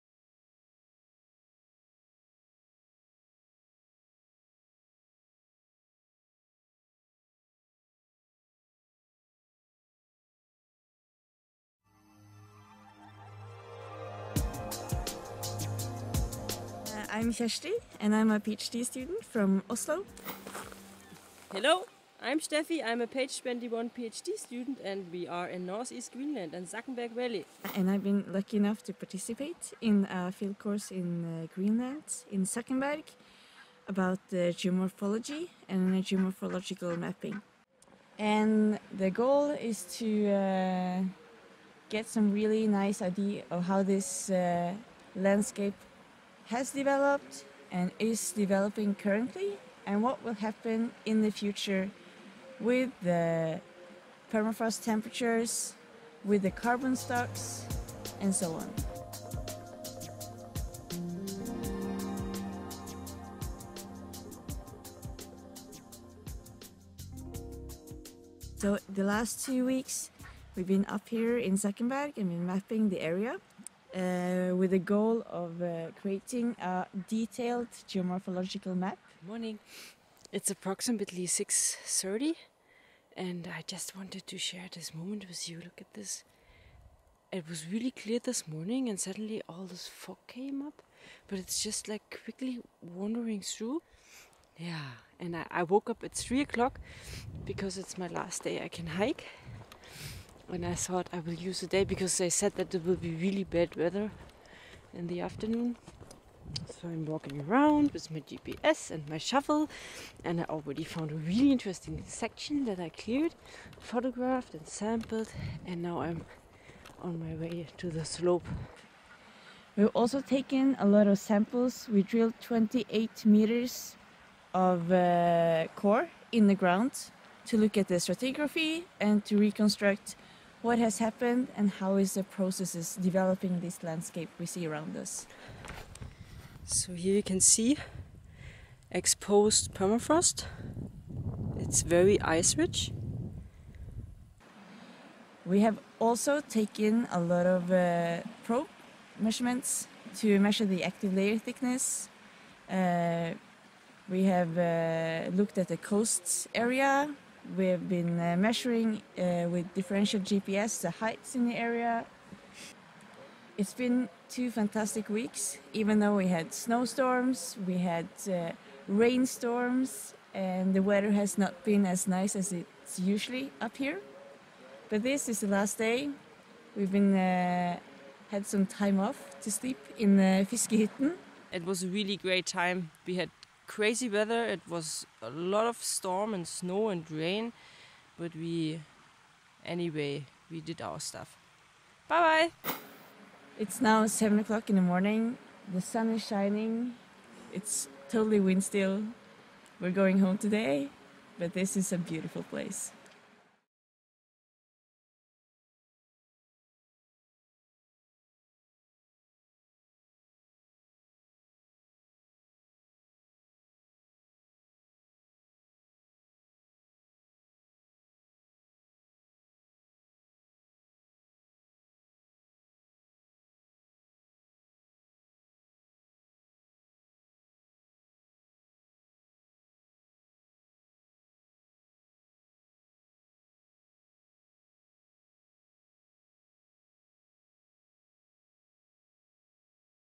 Permafrost research in NE-Greenland - Ten students one goal
This movie shows the fieldwork of ten young permafrost researchers from four Nordic Universities who set out to remote Zackenberg valley at 74 ° N in NE-Greenland. The students participate in a PhD course entitled AG 833 "High-Arctic Permafrost landscape dynamics in Svalbard and NE-Greenland". Goal of this course is to better understand the landscape history and to link present and future periglacial processes with carbon and nitrogen dynamics at two high-Arctic sites: Zackenberg valley in NE-Greenland and Svalbard. AG 833 is organised by the University Centre in Svalbard (UNIS), Norway, and the Center for Permafrost (CENPERM) at the University of Copenhagen, financially supported by Perma-Nordnet of NORDEN, a project as part of the "Nordic Ministerial Council" Arctic collaboration program in Norway.